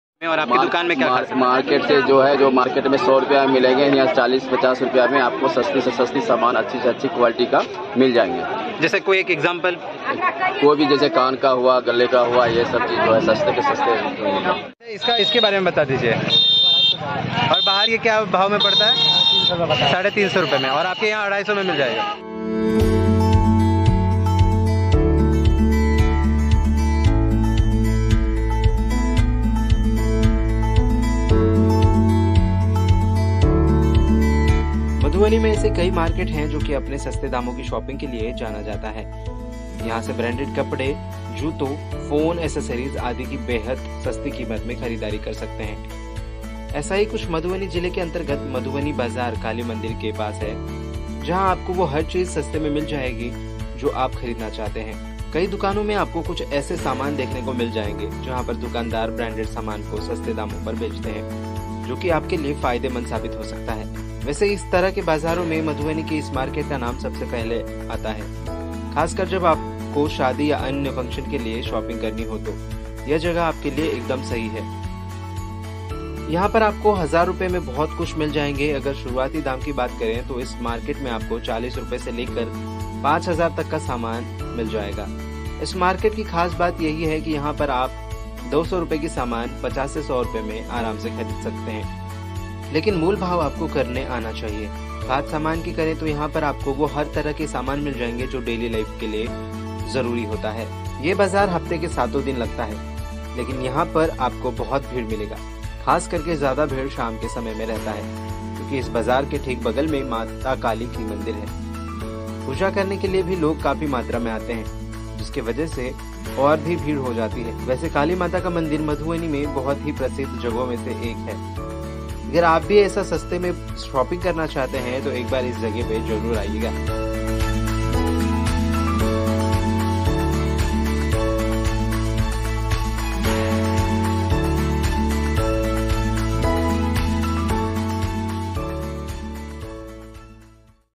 Madhubani News : मधुबनी का ये मार्केट है सबसे सस्‍ता, 40 रुपये में भी कर सकते हैं शॉपिंग, वैरायटी की
राजाराम मंडल/मधुबनी.मधुबनी में ऐसे कई मार्केट हैं जो की अपने सस्ते दामों की शॉपिंग के लिए जाना जाता है. यहां से ब्रांडेड कपड़ों, जूतों, फोन एसेसरीज आदि की बेहद ही सस्ती कीमत में खरीदारी कर सकते हैं. ऐसा ही कुछ मधुबनी जिले के अंतर्गत मधुबनी बाजार काली मंदिर के पास है. जहां आपको वो हर चीज सस्ते में मिल जायेगीजो आप खरीदना चाहते हैं. कई दुकानों में आपको कुछ ऐसे समान देखने को मिल जाएंगे. जहां पर दुकानदार ब्रांडेड सामानों को सस्ते दामों पर बेचते हैं. जो की आपके लिए फायदेमंद साबित हो सकता है. वैसे इस तरह के बाजारों में मधुबनी के इस मार्केट का नाम सबसे पहले आता है. खास कर जब आपको शादी या अन्य फंक्शन के लिए शॉपिंग करनी हो तो ये जगह आपके लिए एकदम सही है.

यहां पर आपको 1000 रु में बहुत कुछ मिल जायेगा. अगर शुरूवाती दाम की बात करें तो इस मार्केट में आपको 40 रु से लेकर 10 हजार तक का बढ़िया समान मिल जायेगा. इस मार्केट की खास बात यही है की यहां पर आप 200 रुपए की चीज को 50-100 में रुपए खरीद सकते हैं. लेकिन मोलभाव आपको करने आना चाहिए. बात समान की करें तो यहां पर आपको हर फंक्शन के हिसाब का कपड़े, जूते मिलतेहैं. ये बाजार हफ्ते के सातों दिन लगता है, लेकिन यह पर आपको बहुत भीड़ मिलेगी. खास करके ज्यादा भीड़ शाम के समय में रहता है. क्योंकि इस बाजार के ठीक बगल में माता काली की मंदिर है. पूजा करने के लिए भी लोग काफी मात्रा में आते हैं. जिसवजहसे और भीड़ हो जाती है. वैसे काली माता का मंदिर मधुबनी में बहुत ही प्रसिद्ध जगहों में से एक है.

कम कीमत पर मिलेगा अच्छा रेंज
चाहे कपड़े हो, एक्सेसरीज़ होया आपकी पसंद के जूते.आपको इस बाजार में हर जरूरत के सामान इतनी कम कीमत पर मिल जाएंगे, जिसकी कल्पना भी नहीं कर सकते हैं. जैसे जूते और हील्स शूज हर तरह की वैरायटी वाले ऑप्शन यहां उपलब्ध हैं. यहां शूज की शुरुआत 500 रुपए से शुरू होती है. इसके अलावा यहां आपको कपड़े में ट्रेडिशनल कपड़े से लेकर वेस्टर्न कपड़े तक मिल जायेंगे. साथ ही घर में उपयोग होने वाली हर तरह की चीज उपलब्ध है.

&nbsp;